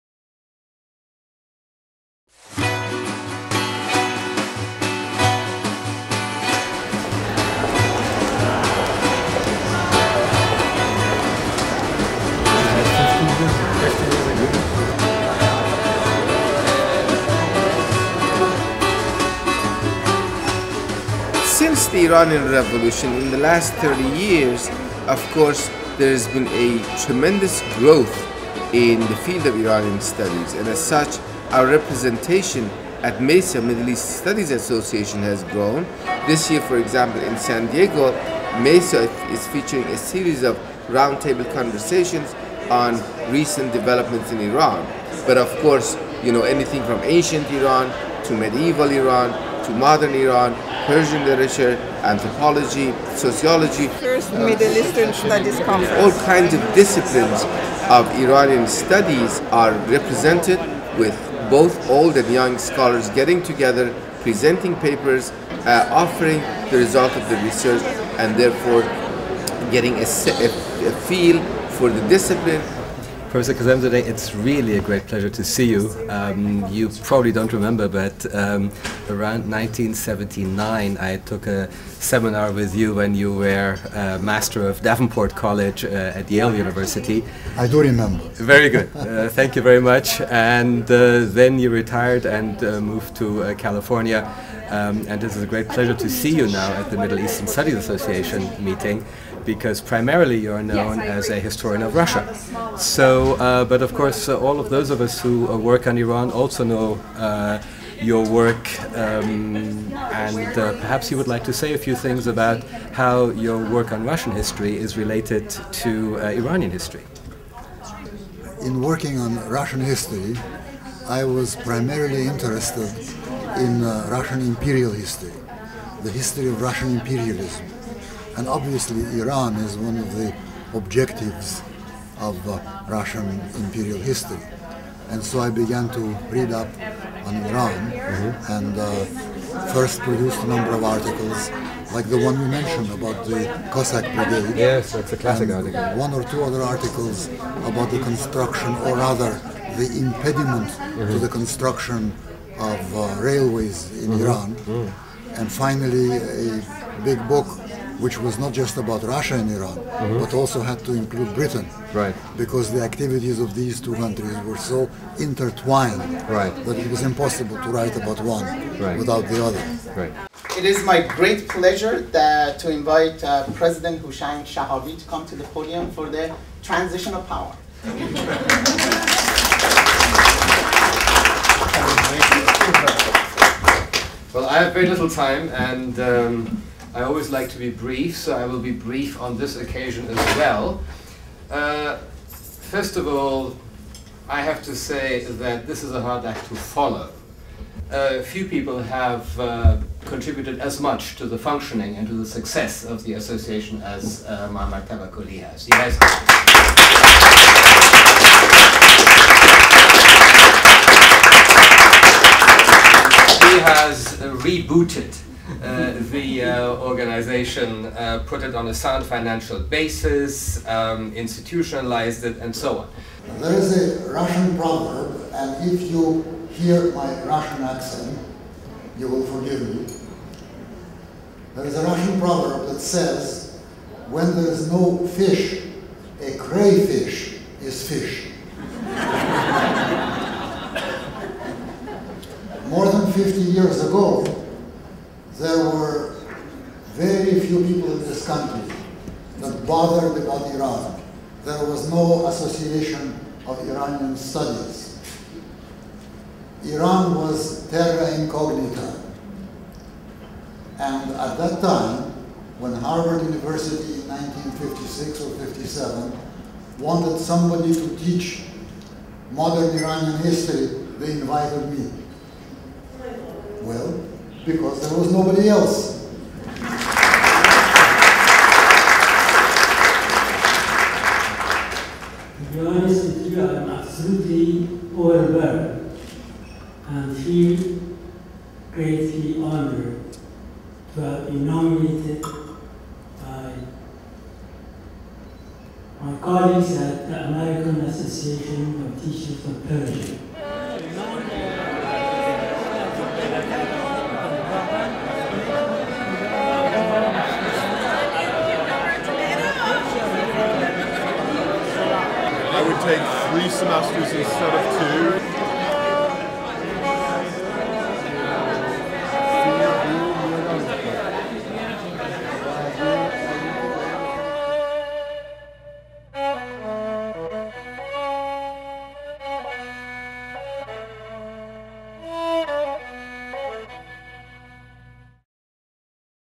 ISIS (International Society for Iranian Studies) at MESA
The International Society for Iranian Studies (ISIS) at the 2010 Middle East Studies Association Conference (MESA) in San Diego.  Highlights include an interview with Dr. Firuz Kazemzadeh by incoming ISIS President, Dr. Houchang Chehabi, and a reception honouring Professor Kazemzadeh and Professor Mehdi Marashi.  The conference took place on November 18-21, 2010.